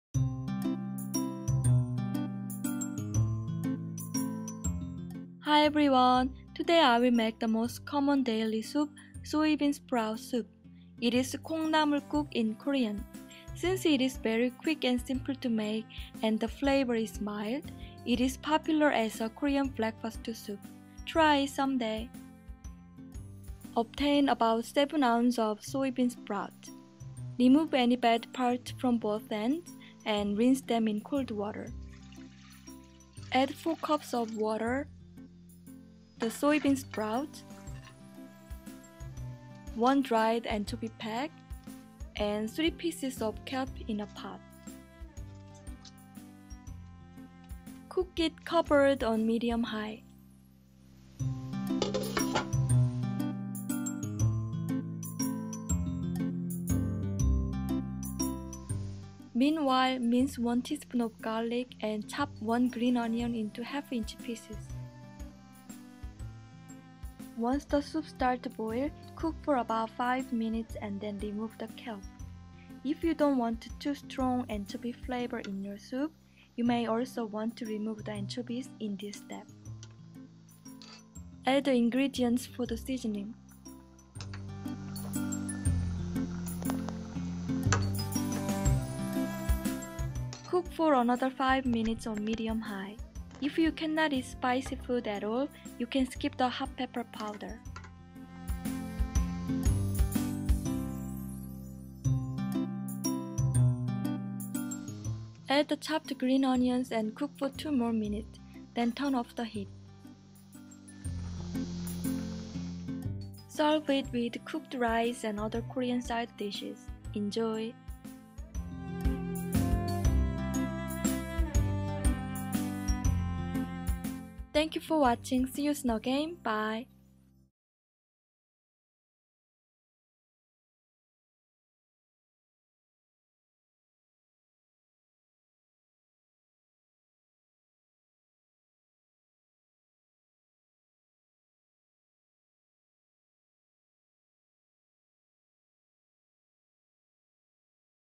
Korean Soybean Sprout Soup (콩나물 국, KongNaMul Guk) | Aeri's Kitchen
👩🏻‍🍳 Soybean sprout guk is the most common daily soup in Korea. Since it is very quick and simple to make, and the flavor is mild, it is popular as a Korean breakfast soup. Korean people say that it is also good for a hangover, but I cannot attest to that. There are different versions of soybean sprout soup; this recipe is a basic version. Try it someday.

      Love,
      Aeri 😊